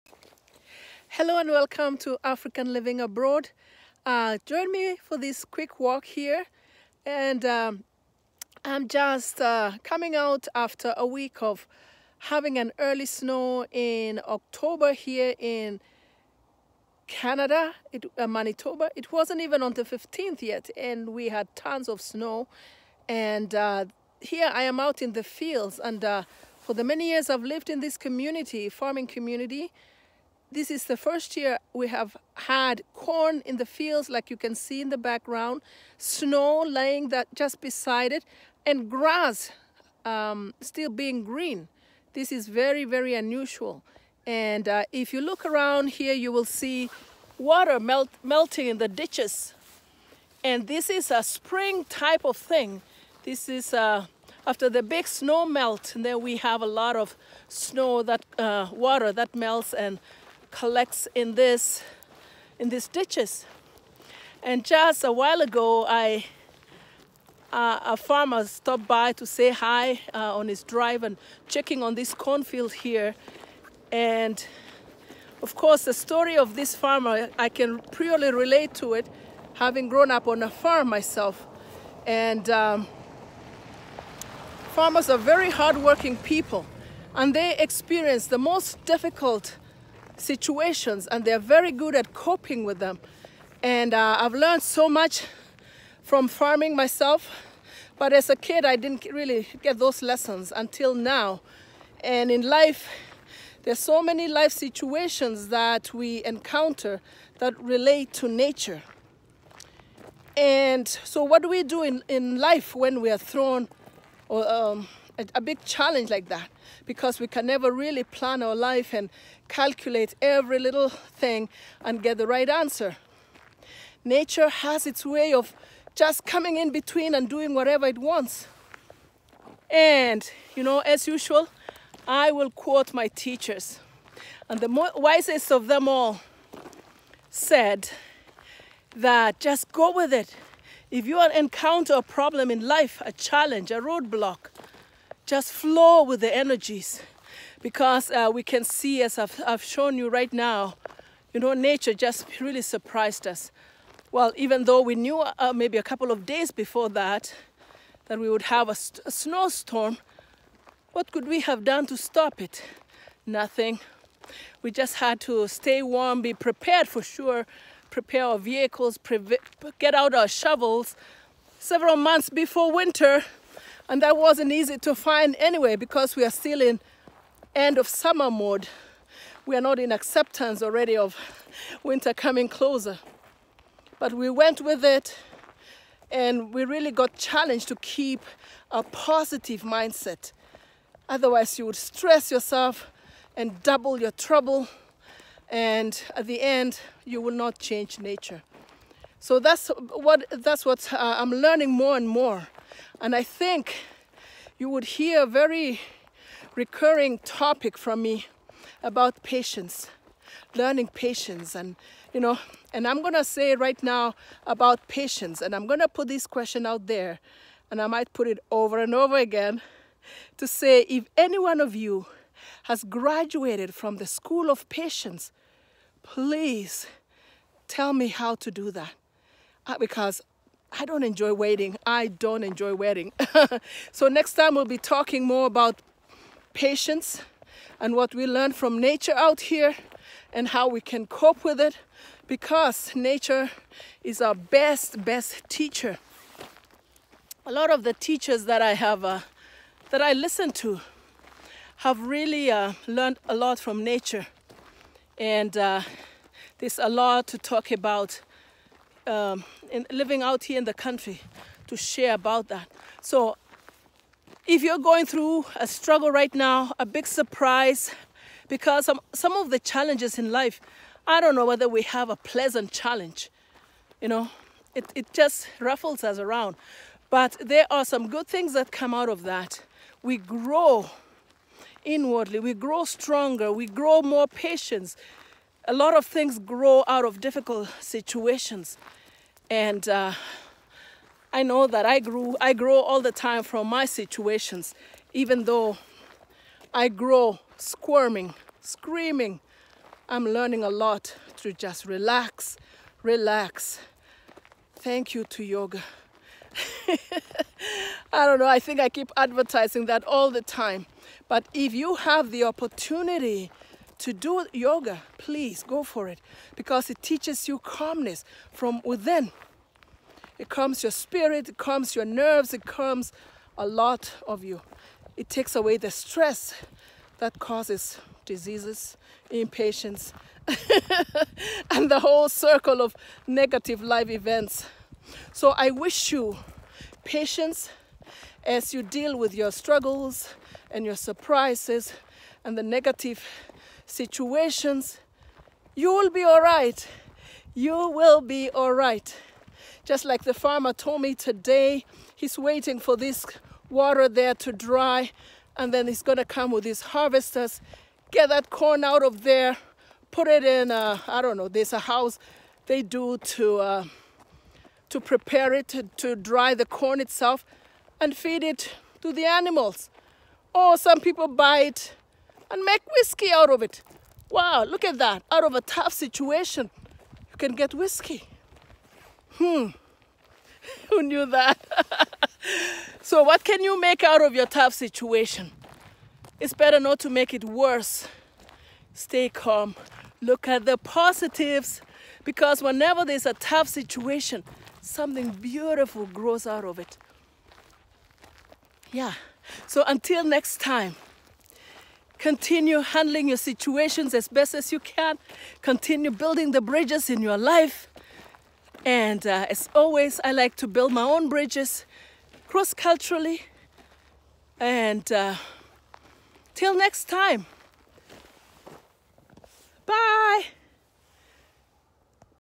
Can you make whisky out of your challenges ?
How do you handle personal emergencies? Staying calm and accepting inevitable challenges in life could be a big help. Learn to be  patient and allow the situation to  unfold and  to pass.
But being prepared with the info at hand  is wise.